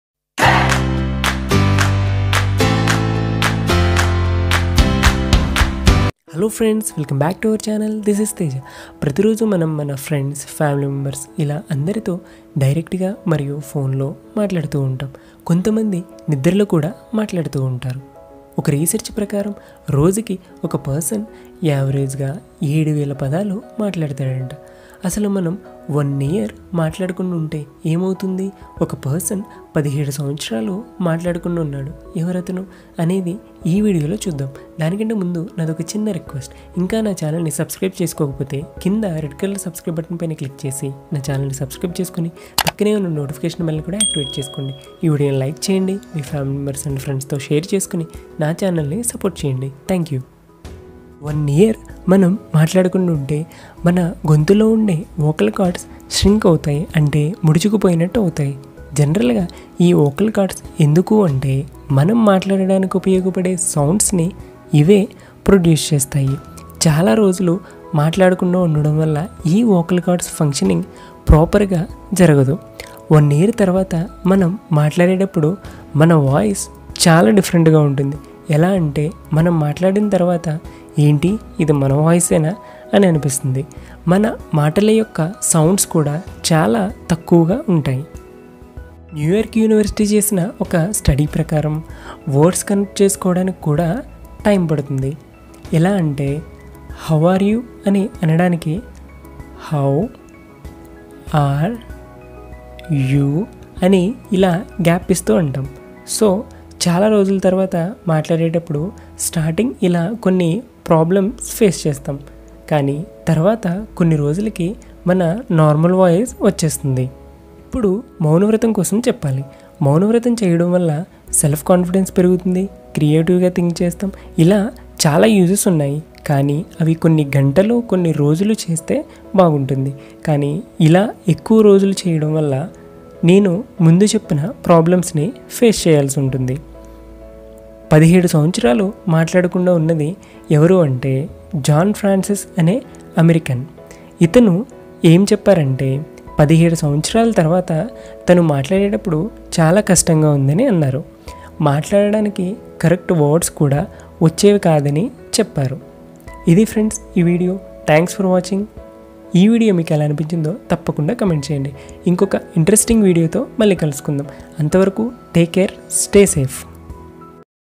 What happens if you stop talking for a year? ఒక సంవత్సరం మాట్లాడకుండా ఉంటే ఎం అవుతుంది?
I DISCUSSED IN THIS VIDEO :

What happens if you stop talking for a year?
who is the person didn't speak for 17 years?